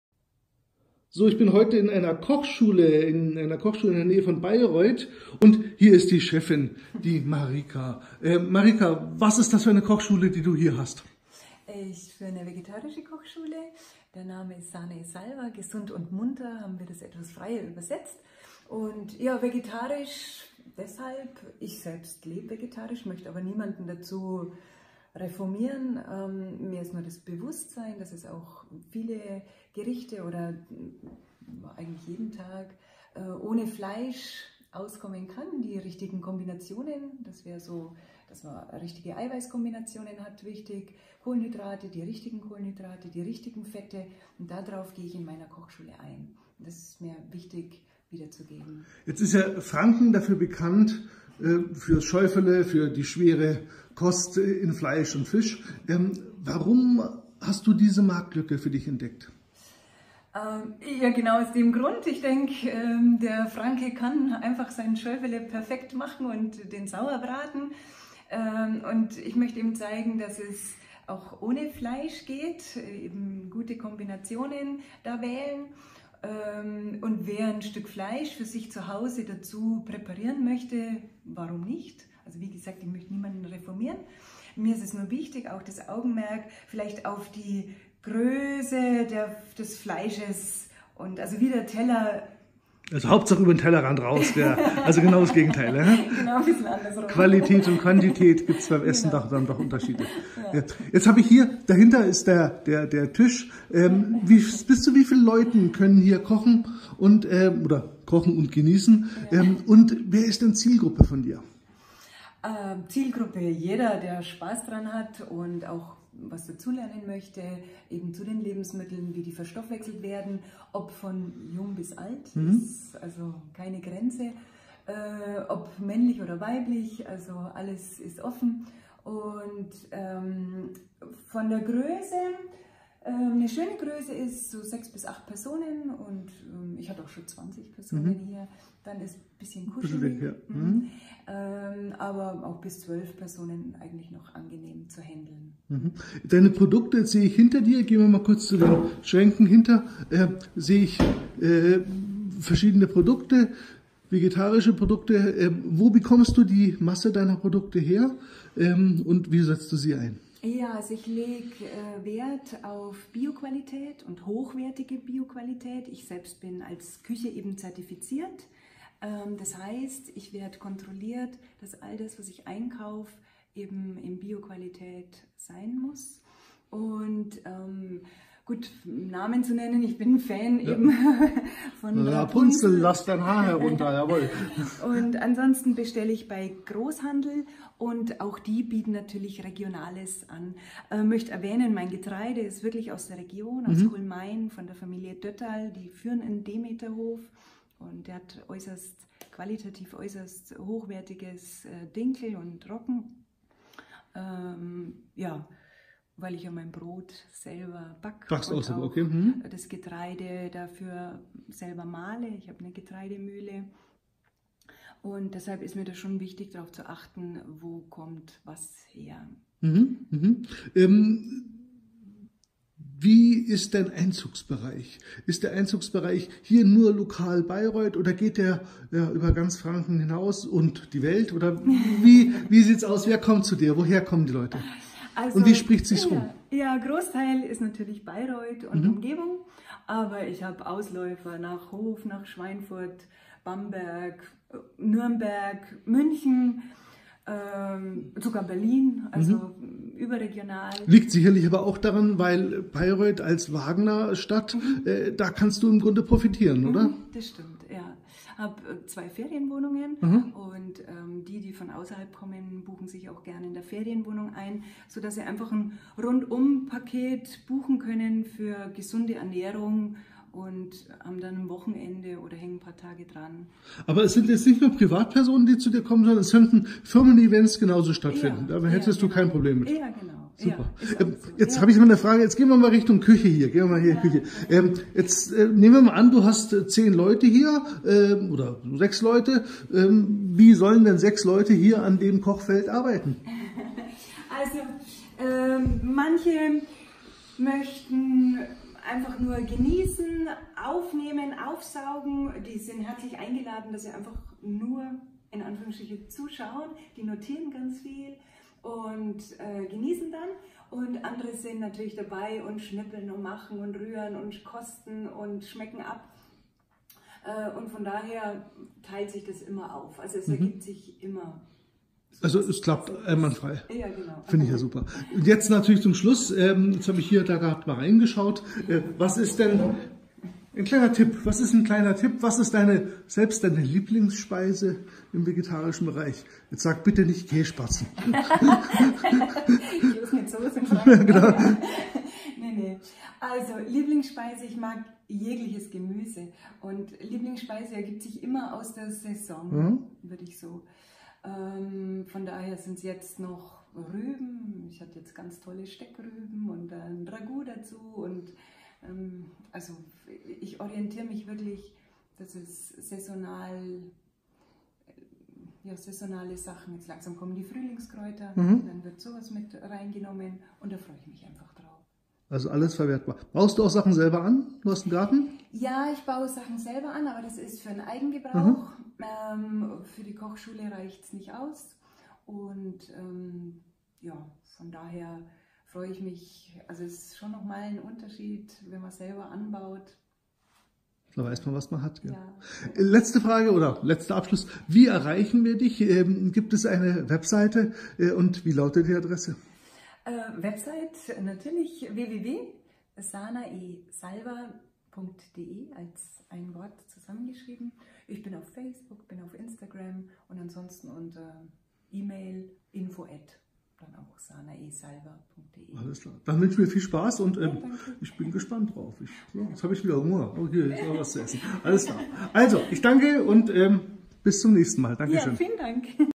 Vegetarische Kochschule Sana e Salva bei Bayreuth
Marika Bange leitet die Kochschule Sana e Salva in Eckersdorf in der Nähe von Bayreuth. Bei ihr gibt es ausschließlich leckere vegetarische Küche. Matthias J. Lange von redaktion42 war bei Marika Bange zu besuch und hat sich das Konzept einer vegetarischem Kochschule erklären lassen.